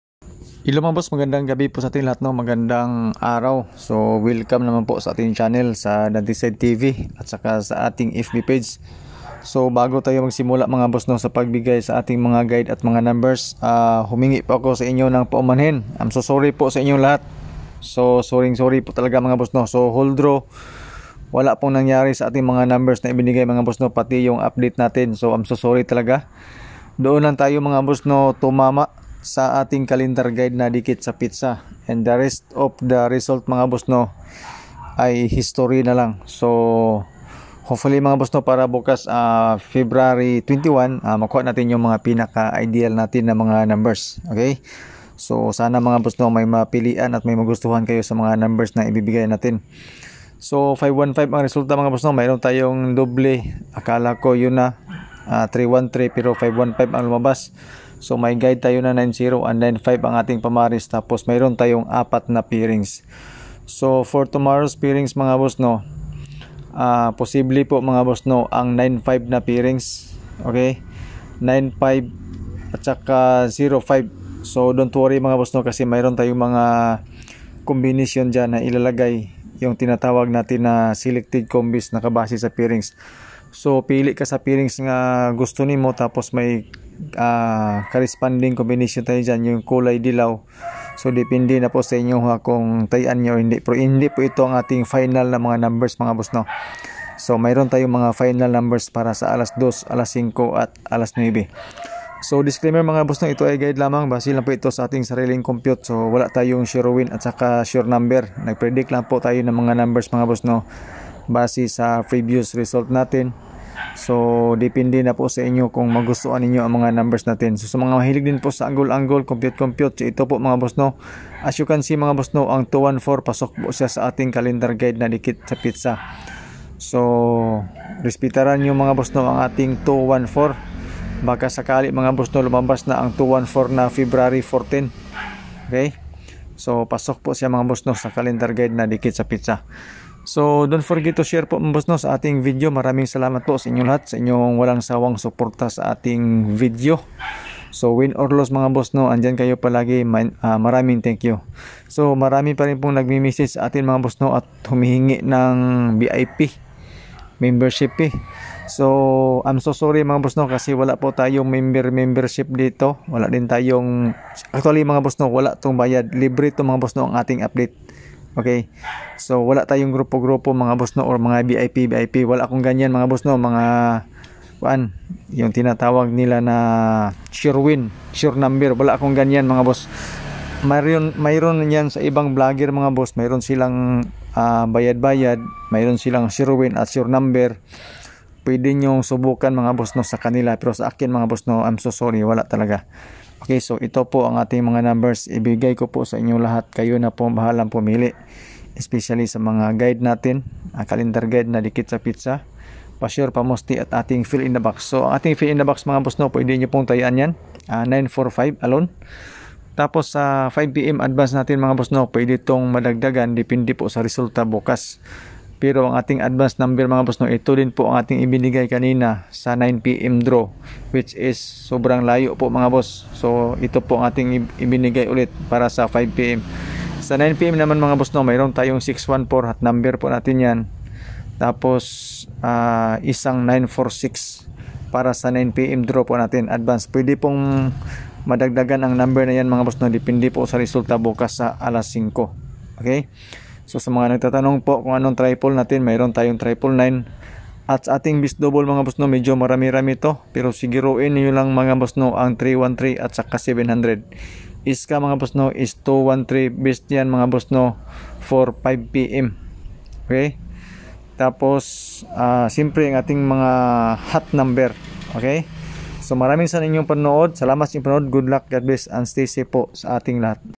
(1 combi hearing) Best pair, 61,46 plus bonus number SWERTRES 3DLOTTO AND STL. FEBRUARY 21 2024
Swertres and stl guide pairings hearing probables ngayon base sa previous result.